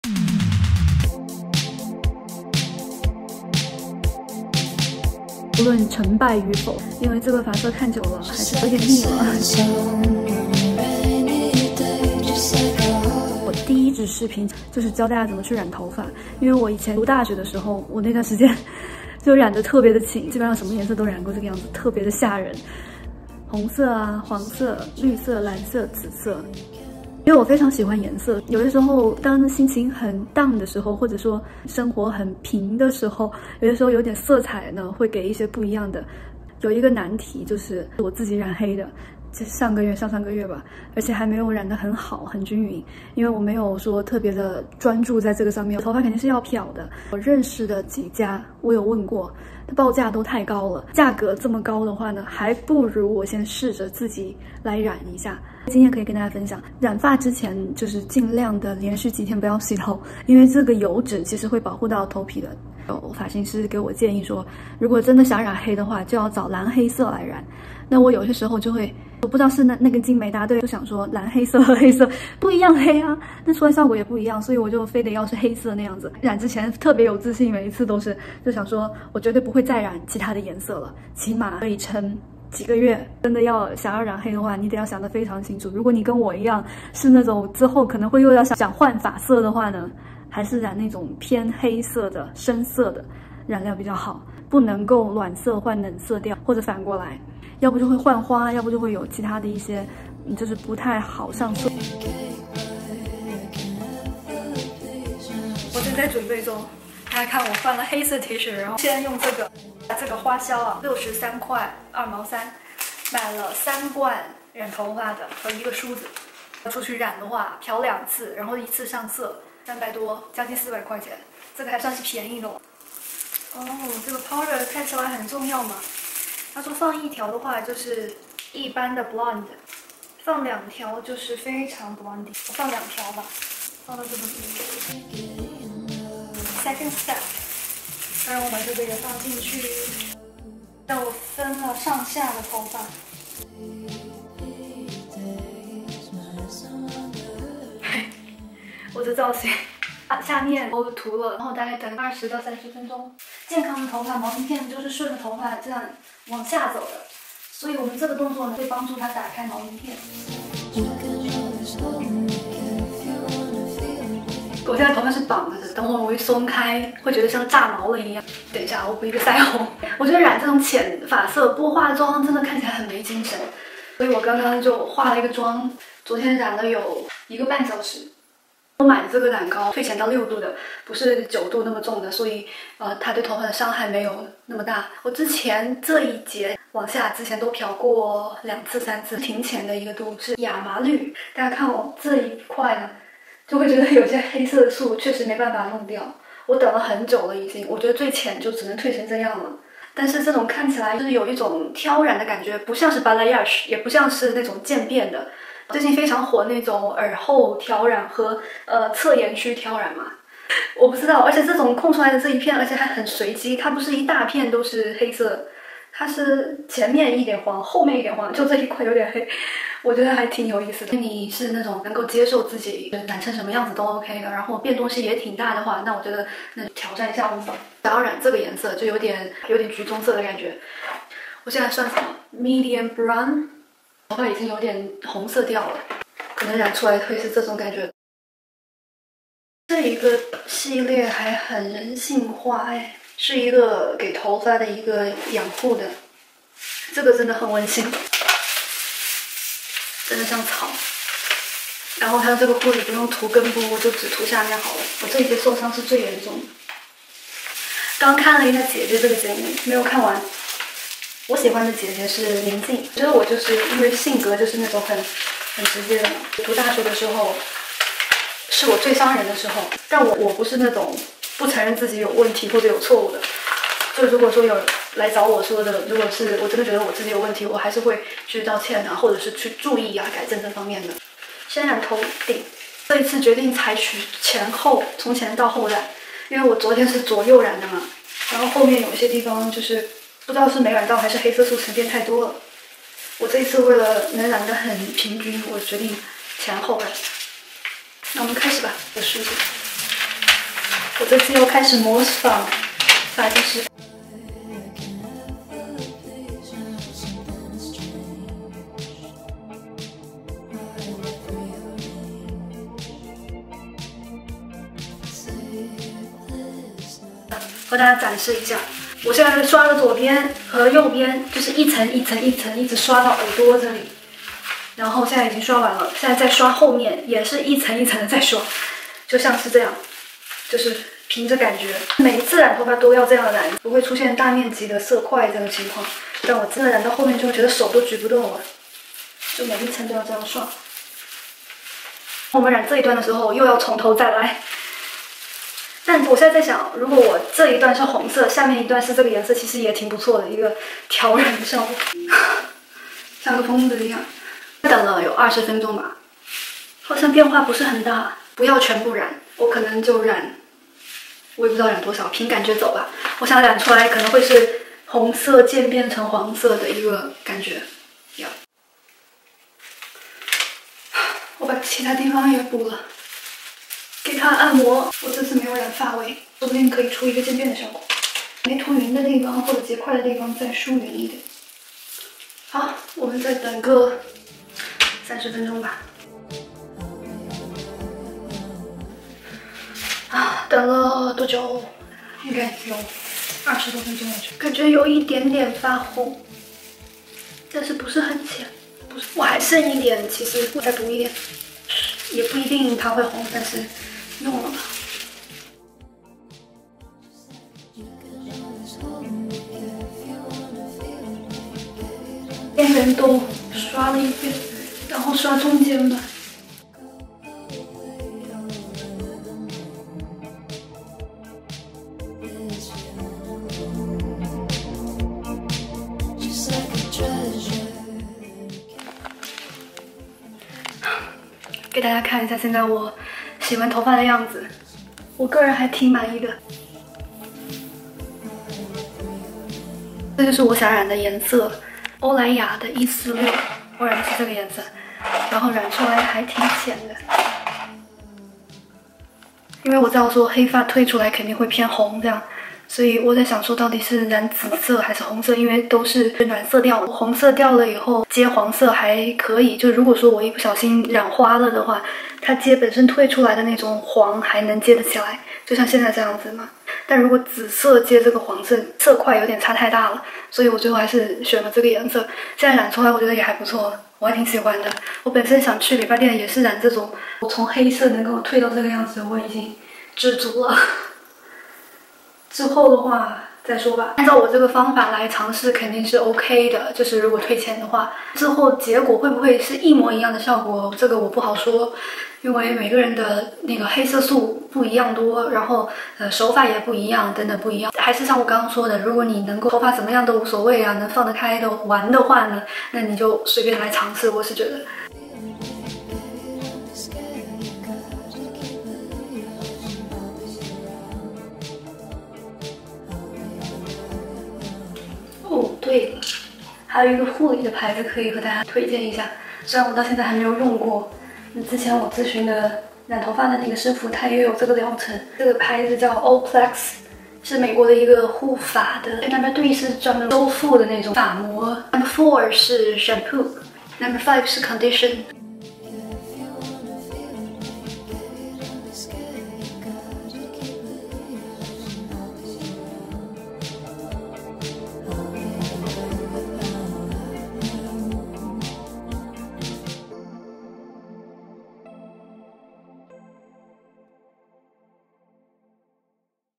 如何自己漂发2.0|my hair transfomation part I|how to bleach&dye hair|LaMichelle
halou各位，
非常random的一次褪浅头发的挑战，我的经验是之前去染发的时候观察来的，不是专业的，如果你很随意，欢迎和我一起来试试。😊

🦋Product
L'Oreal Paris Perfect Blondissima

L'Oreal Paris Excellence Crème Permanent Hair Colour E46 Copper Red Brown

🦋0:09 BACKGROUND
🦋0:55 TIPS
🦋1:41 BLEACHING HAIR
🦋2:40 DYEING HAIR
🦋7:04 AFTER RINSING HAIR
🦋8:24 STYLING HAIR

微博：十万个雪仔
小红书：LaMichelle
抖音：LaMichelle